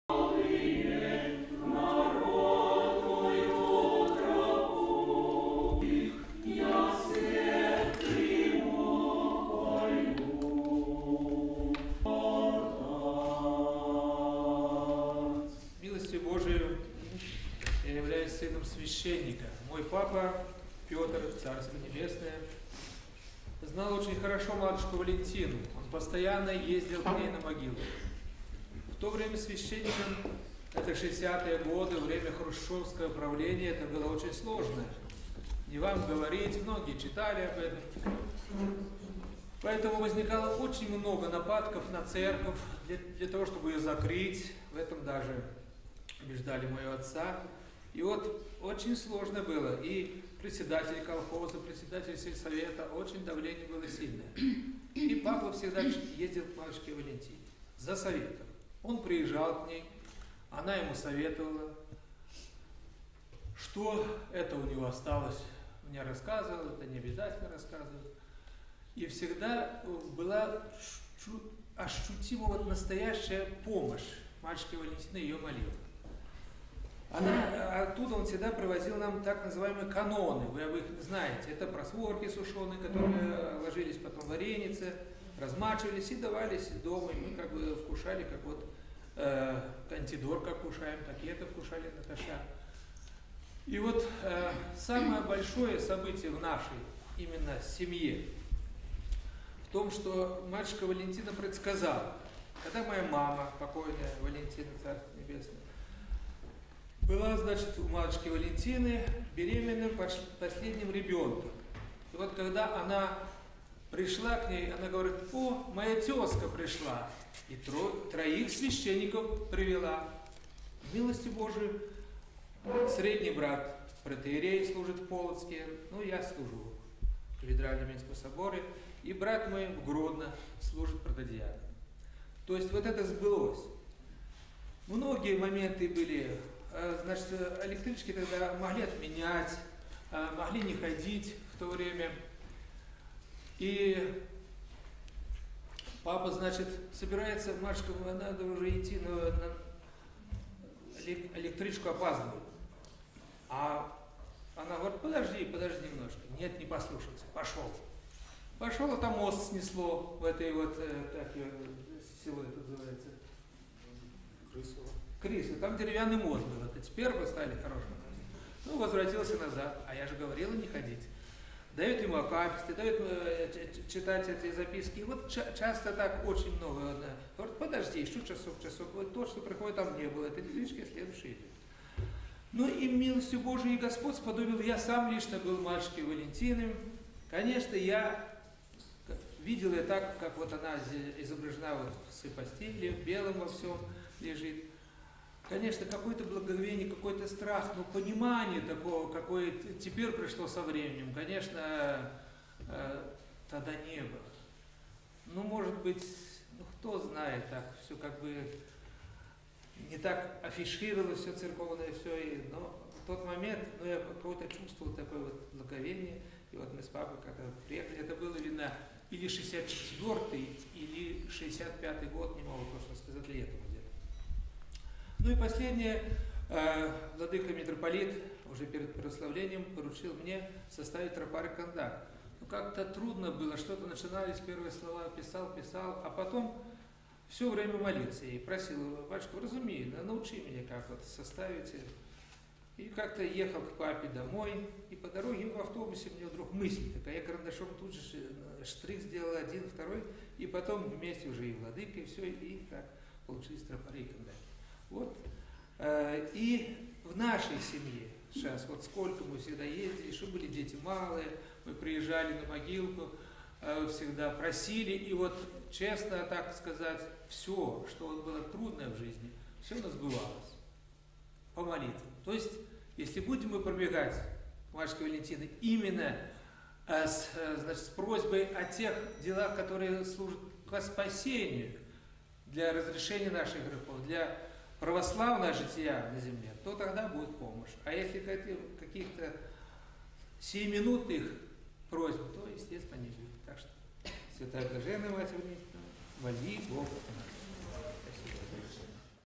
"Матушка Валентина предсказала мне священство"
Открытие выставки «Народом нареченная святой», которое состоялось 26 января в Свято-Покровском храме Минска, собрало многих свидетелей помощи святой блаженной Валентины Минской. На этой встрече особенно интересно было услышать рассказы тех, кто знал матушку Валентину во времена её земных дней, тех, чья жизнь протекала при её молитвенном предстательстве. Одним из таких людей, кому посчастливилось видеть матушку, быть свидетелем её прозорливости, является клирик Свято-Духова кафедрального собора Минска протодиакон Николай Авсиевич